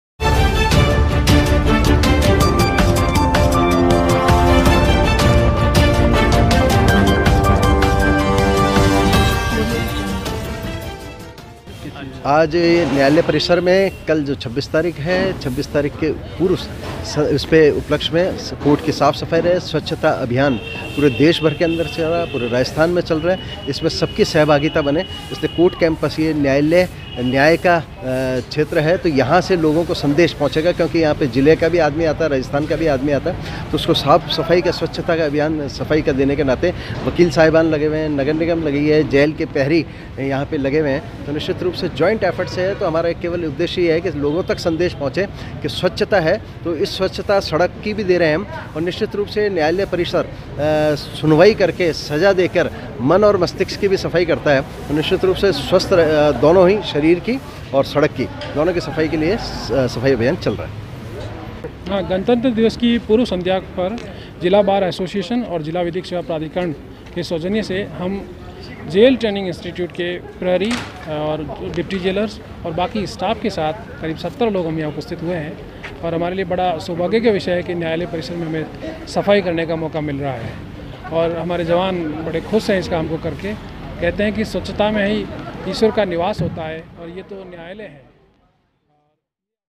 AYN NEWS गणतंत्र दिवस पर जेल ट्रेनिंग इंस्टिट्यूट के पैरी स्टाफ ने न्यायालय परिसर में की सफाई
गणतंत्र दिवस की पूर्व संध्या में जिला बार एसोसिएशन के सौजन्य से जेल ट्रेनिंग इंस्टिट्यूट के पहरी स्टाफ ने न्यायालय परिसर में की सफाई।
जेल ट्रेनिंग इंस्टिट्यूट के पैरी स्टाफ ने न्यायालय परिसर में की सफाई